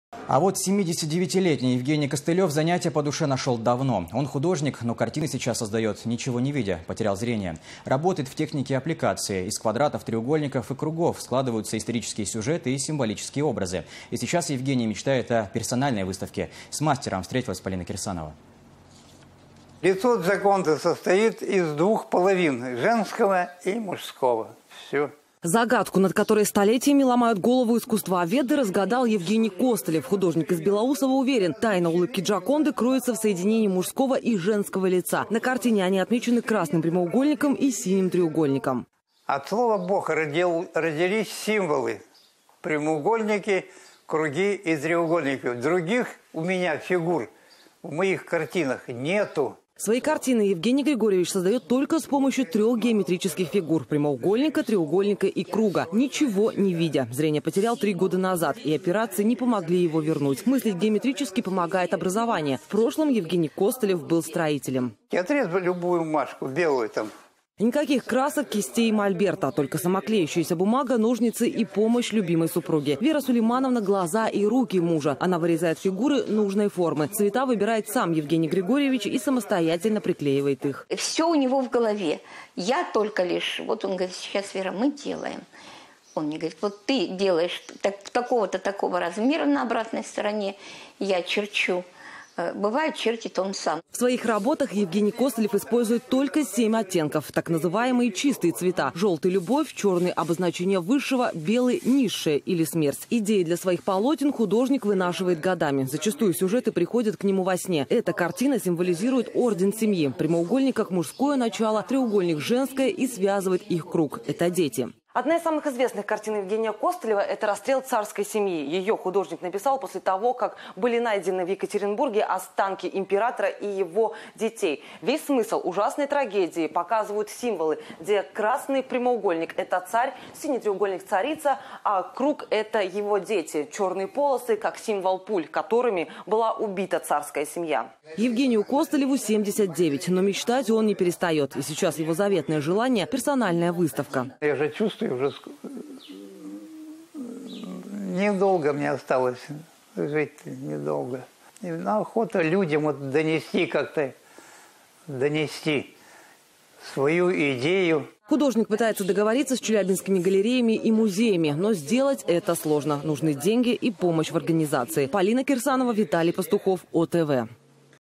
Ослепший художник создает картины-аппликации
79-летний Евгений Костылев давно нашел занятие по душе: он художник, но сейчас создает картины, ничего не видя — потерял зрение. Мужчина работает в технике аппликации и мечтает о персональной выставке. С мастером встретилась корреспондент ОТВ Полина Кирсанова.

Свои картины Евгений Григорьевич создает только с помощью трех геометрических фигур: прямоугольника, треугольника и круга. Зрение потерял три года назад, и операции не помогли его вернуть. Мыслить геометрически помогает образование — в прошлом Евгений Костылев был строителем.

Никаких красок, кистей и мольберта. Только самоклеющаяся бумага, ножницы и помощь любимой супруги. Вера Сулеймановна — глаза и руки мужа. Она вырезает фигуры нужной формы. Цвета выбирает сам Евгений Григорьевич и самостоятельно приклеивает их.

«Все у него в голове. Он говорит: "Сейчас, Вера, будем делать! Вырежи мне деталь такого-то размера". Бывает, чертит он сам», — рассказывает жена художника Вера Костылева.
В своих работах Евгений Костылев использует только 7 оттенков. Так называемые чистые цвета: желтый — любовь, черный — обозначение высшего, белый — низшее или смерть. Идеи для своих полотен художник вынашивает годами. Зачастую сюжеты приходят к нему во сне. Например, одна из картин символизирует орден семьи. Прямоугольник — как мужское начало, треугольник — женское. И связывает их круг — это дети.

Одна из самых известных картин Евгения Костылева — это расстрел царской семьи. Он написал её, когда в Екатиринбурге были найдены останки императора и его детей. Весь ужас этой трагедии в символах, где красный прямоугольник — это царь, синий треугольник — царица, и круг — их дети. Черные полосы — обозначение пуль, которыми была убита царская семья.

Евгений Костылеву 79. Но мечтать он не перестает. И сейчас его заветное желание — о персональной выставке.

«Я уже чувствую, что мне недолго осталось жить, недолго. Охотно людям донести свою идею», — подчеркивает художник.

Евгений Костылев пытается договориться с челябинскими галереями и музеями. Но сделать это сложно. Нужны деньги и помощь в организации.